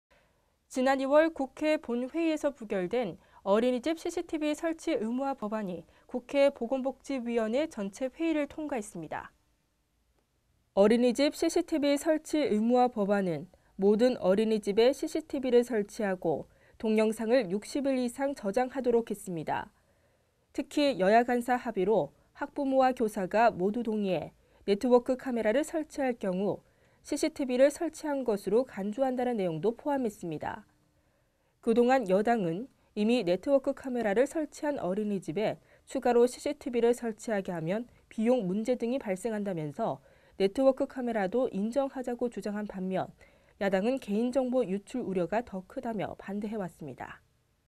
어린이집 CCTV 설치 의무화법안 복지위 통과
지난 2월 국회 본회의에서 부결된 '어린이집 CCTV 설치 의무화 법안'이 국회 보건복지위원회 전체회의를 통과했습니다.

어린이집 CCTV 설치 의무화 법안은 모든 어린이집에 CCTV를 설치하고 동영상을 60일 이상 저장하도록 했습니다.

특히, 여야 간사 합의로 학부모와 교사가 모두 동의해 네트워크 카메라를 설치할 경우 CCTV를 설치한 것으로 간주한다는 내용도 포함했습니다.

그동안 여당은 이미 네트워크 카메라를 설치한 어린이집에 추가로 CCTV를 설치하게 하면 비용문제 등이 발생한다면서 네트워크 카메라도 인정하자고 주장한 반면, 야당은 개인정보 유출 우려가 더 크다며 반대해 왔습니다.